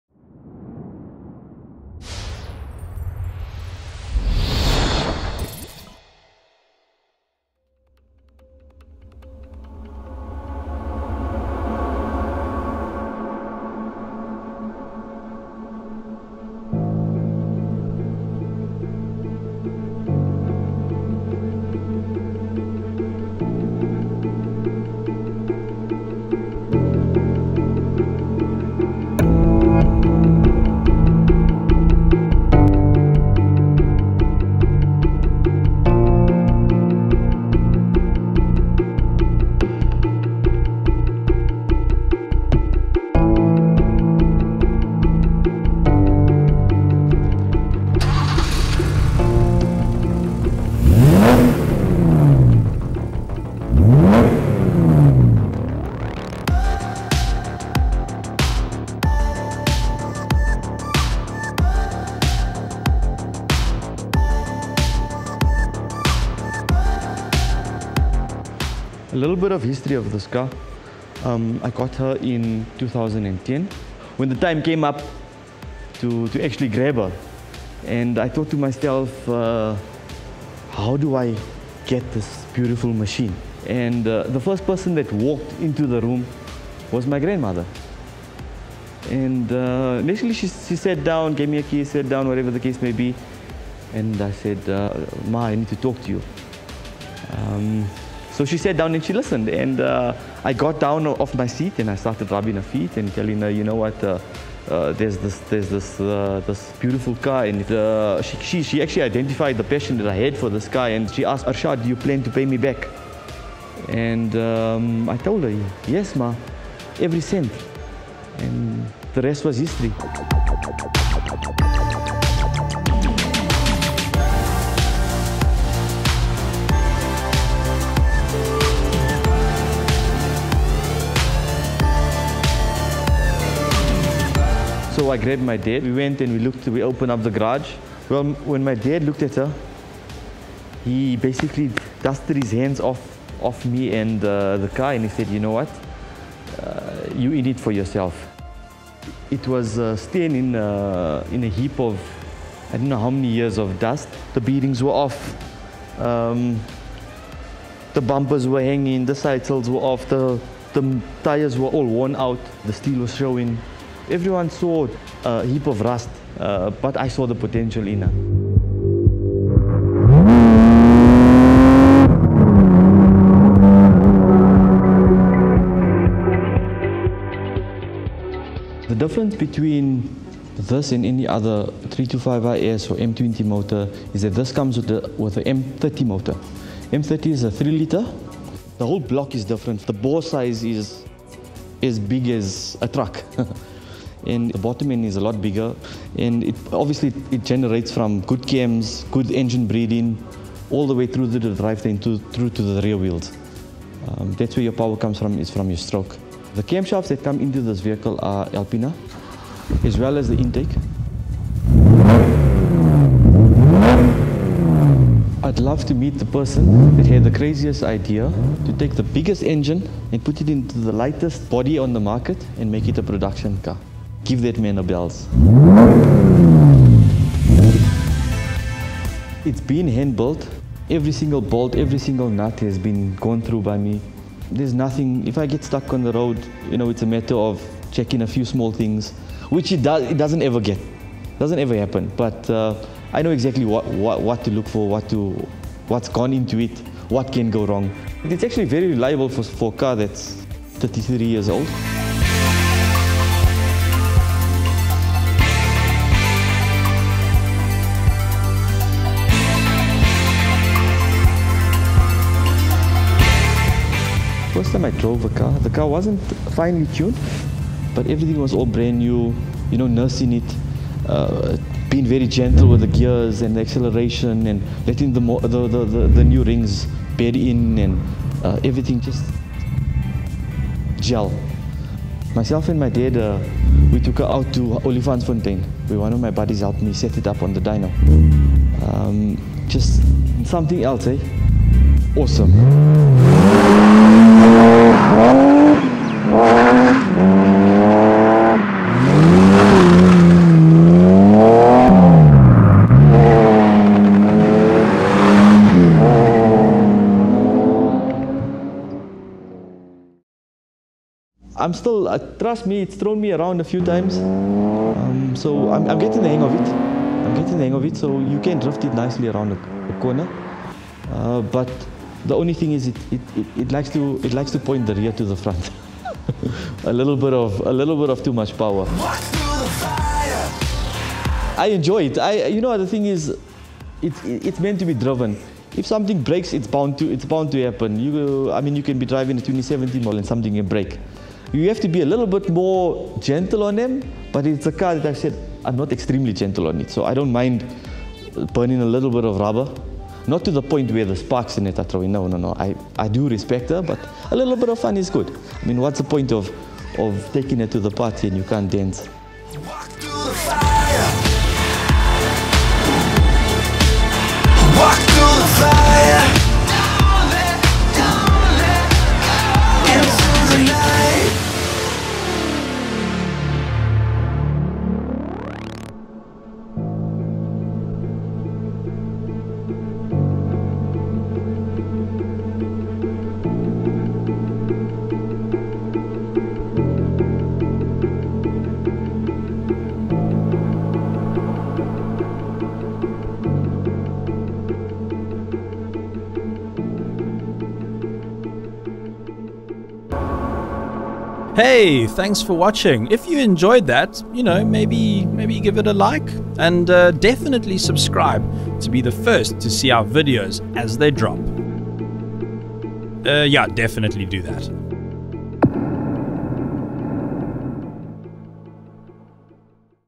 South Africa's BMW 333i - Restoring one of the Rarest and most desirable 3 Series E30s in the world
SentiMETAL Ep 4: White Gold
The emotional story of Arshaad's rebuild of his gorgeous BMW 333i in South Africa. The iconic 333i is a South African special - one of a few cars that were built exclusively for the South African market. Arshaad restored his BMW from a heap of rust to the incredibly beautiful one of a kind 333i, aptly named White Gold. With so few of these incredible cars left, restorations like this help preserve the heritage of this rare and amazing machine.

The story of the 333i:

In the mid-80s BMW fanatics were enthralled by the debut of the first-ever M3, but the iconic left-hand-drive-only sportscar (now highly sought-after by collectors) was not destined for South Africa… Local enthusiasts such as Lenasia’s Arshaad Nana could only dream of owning one.

However, BMW South Africa partnered with BMW Motorsport and Alpina to develop a uniquely South African E30 edition, called the 333i. It took the M30 engine from the 733i and shoehorned it into the engine bay of the small and light 3 Series, instantly creating one very hot compact sedan.

Only around 200 cars were sold to the South African public, making the 333i one of the rarest and most desirable E30s in the world. Key to its appeal was always the stirring straight-6 under the bonnet, as opposed to the 4-cylinder used in the M3. In the 333i, and with the help of various Alpina bits, the 3.2-litre motor punched out 148 kW, which doesn’t sound like much today, but the low-down torque delivery, as well as its stirring soundtrack and edgy dynamics make the 333i a massively rewarding car to drive.

Owner Arshaad Nana, who owns BMW specialist workshop EuroSpec, was entertained by the 325iS models on South Africa’s racing circuits in his youth, but for him, the 333i was always the ultimate – it was his destiny to own one.

“I found the car, or the car found me, a very long time before I could buy it,” he explains. Given the growing value and desirability of the 333i in recent years, Arshaad knows he was lucky that it wasn’t snapped up before 2010, when he finally was ready to buy it, with help from his grandmother. Neglected for years and standing under dust, Arshaad bought the car and lovingly restored it to its current state himself. “I know every nut and bolt in this car,” he says.

0:00 Intro
2:58 Comparison to BMW 325is
4:19 Driving experience
5:15 Handling
6:01 Outro

Featuring: Ashraad Nana - BMW 333i
Directed By: Justin Pinto
Story and Research: Hannes Oosthuizen, Jenna Mervis
2nd camera and Gimabal Operator: Shaan Bester
Assistant : Riaan Fourie
Online: Sanchez Labs
Filmed on location in Johannesburg, South Africa